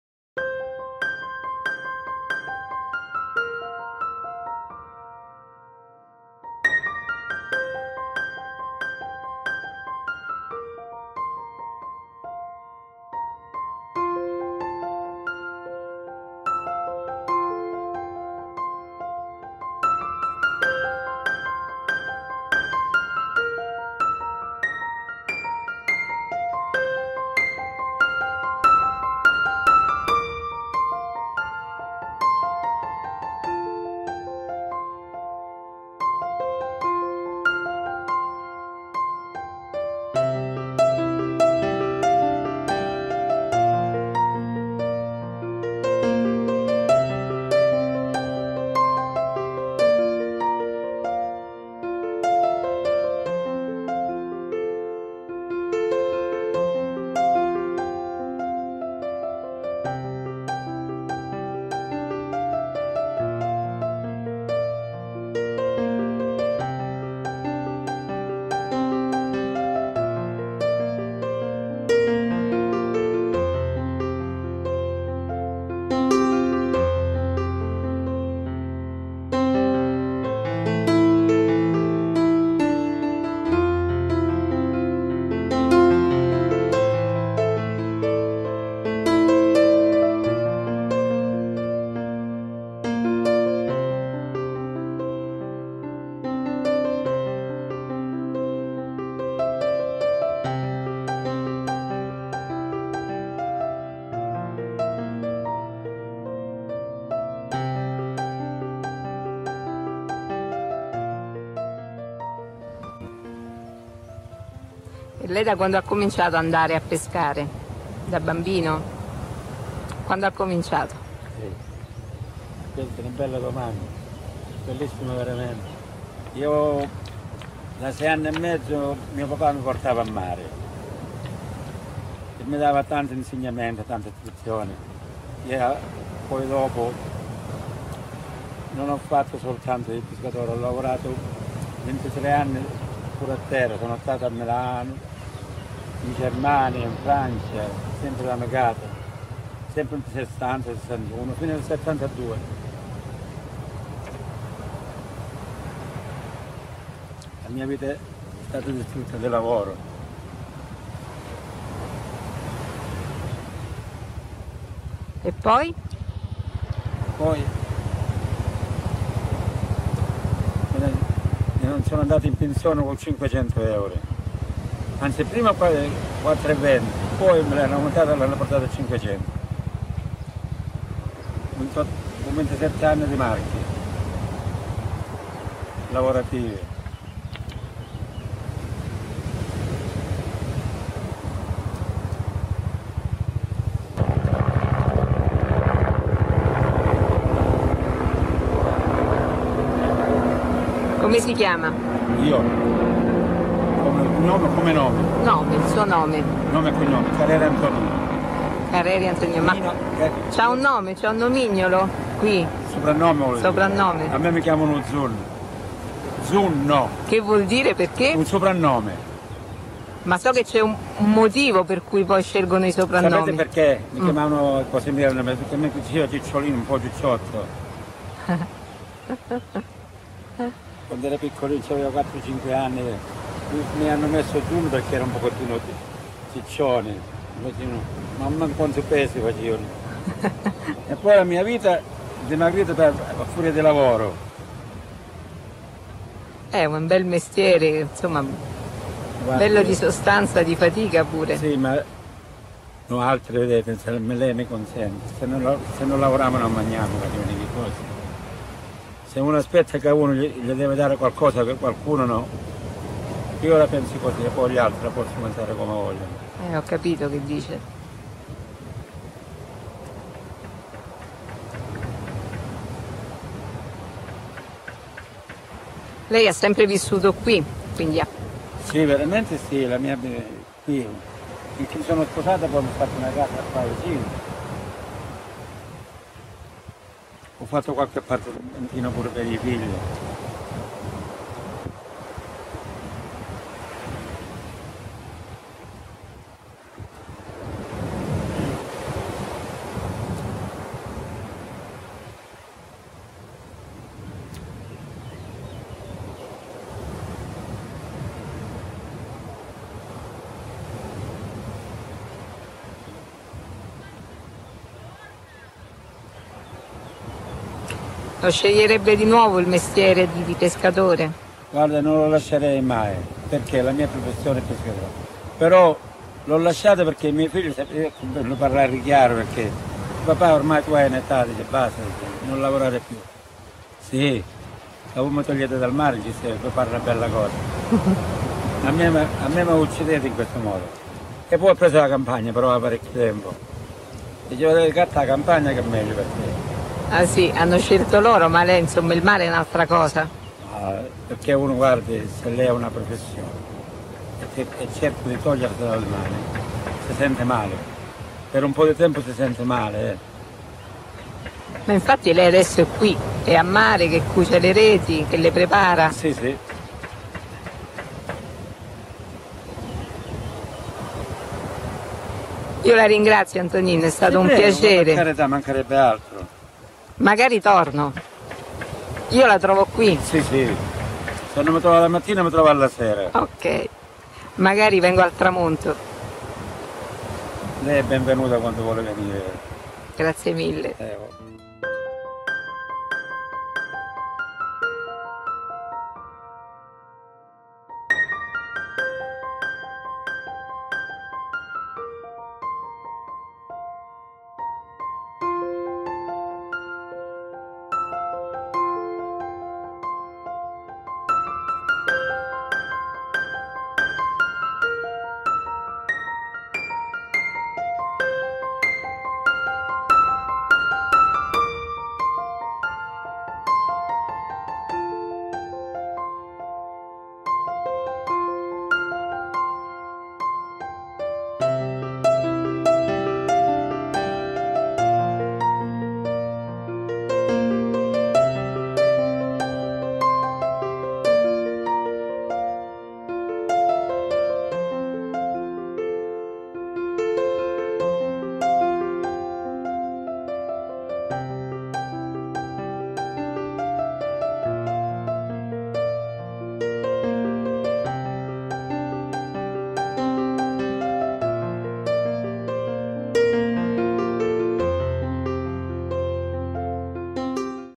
Chatting with a fisherman
My history with the fishermen begins at «la Tonnara». I have no words to describe what I feel every time I see it. I only know that seeing the fishermen’s huts on the beach, where they use to stay until late at night, almost as if they will never leave, soothes my mind and my body. It is a whole.
Antonino was the first encounter, of warmth and Human Beauty.